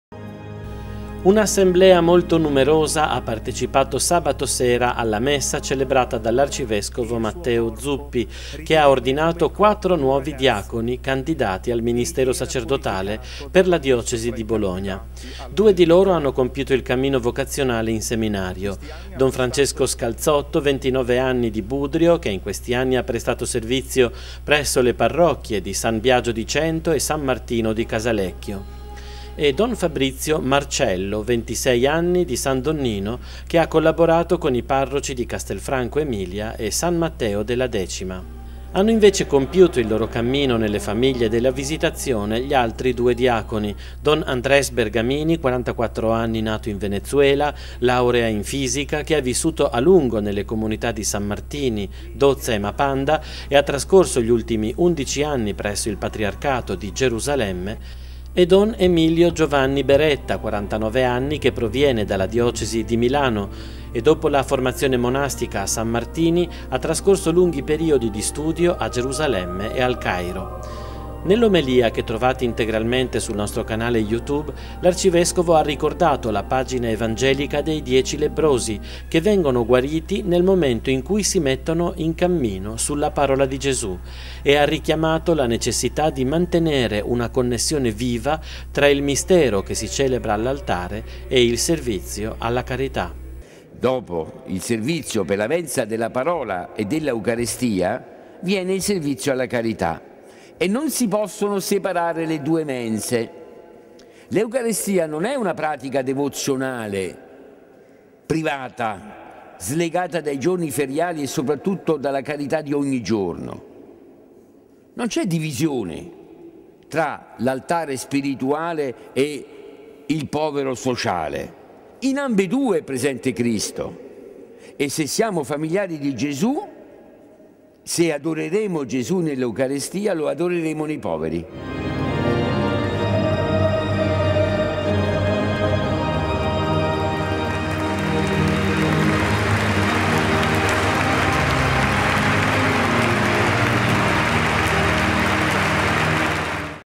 4 diaconi candidati al sacerdozio
12PORTE - 16 ottobre 2016: Il sabato dopo San Petronio: ritorna l’appuntamento carico di speranza per la Chiesa bolognese delle ordinazioni dei diaconi candidati al sacerdozio. Sono 4 i giovani che hanno ricevuto il primo grado dell’ordine sacro dall’Arcivescovo.